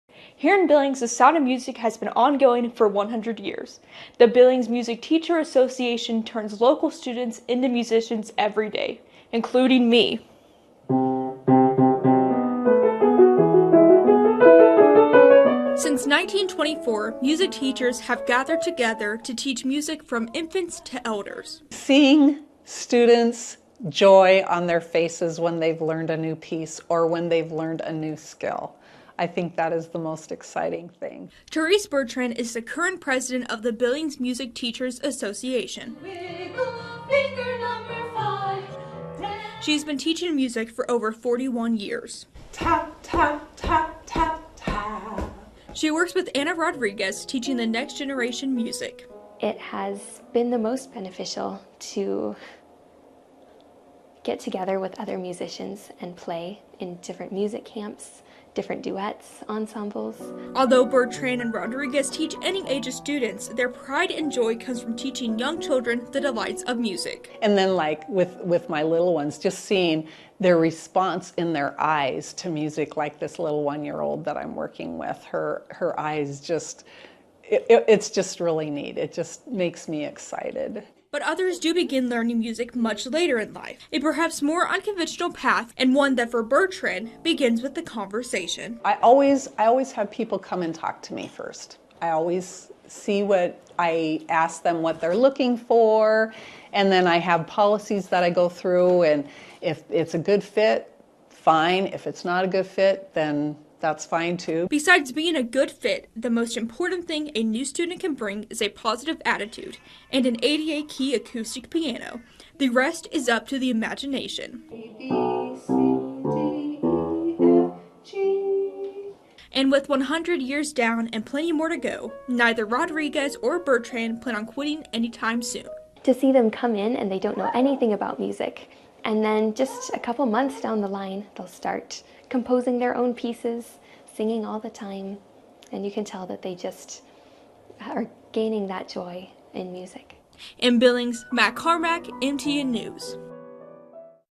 100 years of music at the Billings Music Teacher Association
Here in Billings, the sound of music has been ongoing for 100 years.   The Billings Music Teachers Association (BMTA) has been turning local students into musicians since 1924, including me.   Almost most students begin at a young age, the Association teaches anyone from age 0 to 100.